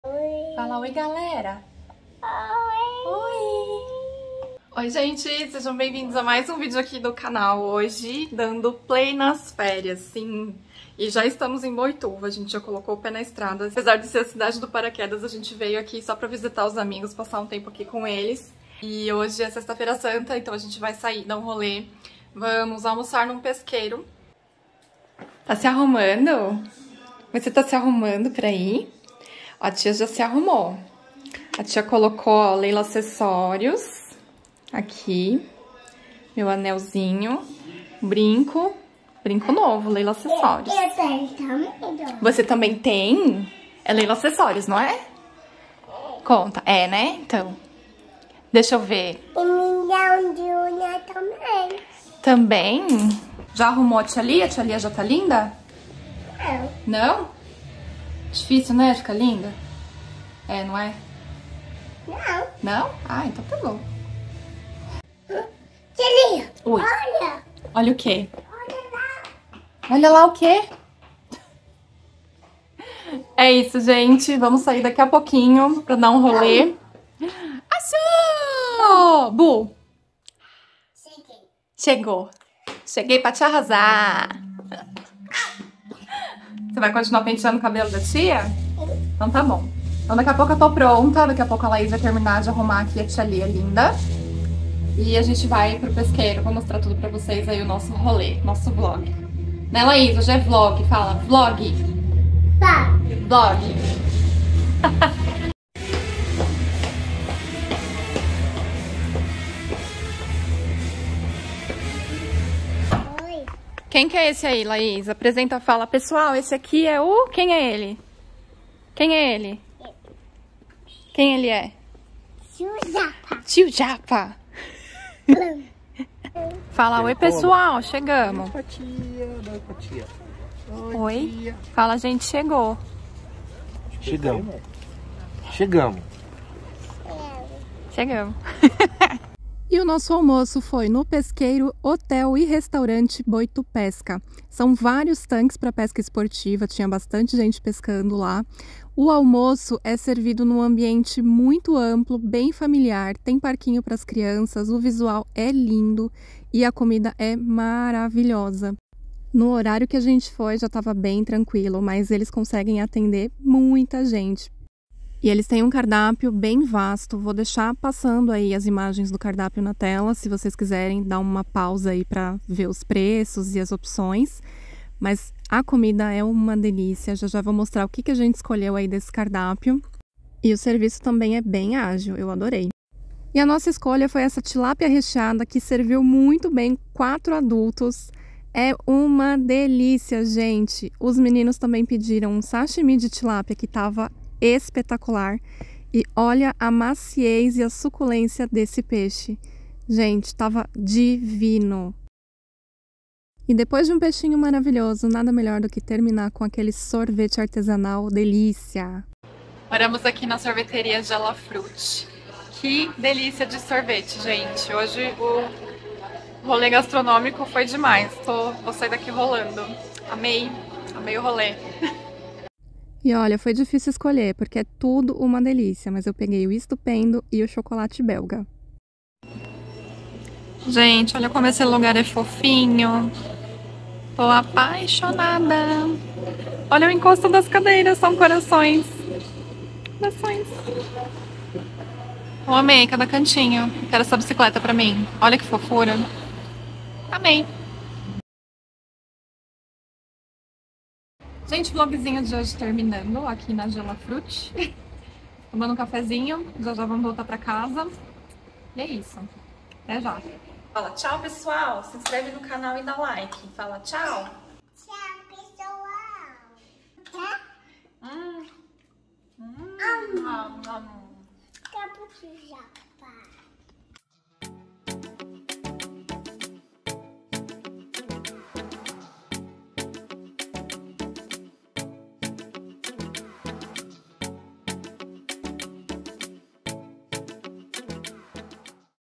Vlog de férias!! Boituva| O vídeo mais fofo do canal 💕
Vloguei em Boituva dando play nas férias! Nesse vídeo contei com a participação especial da Laís, filha dos nossos amigos, ela que deu a graça e fofura pra esse vídeo. Tia Lia e tio japa amam 💕
Foi bom demais começar as férias em boa companhia.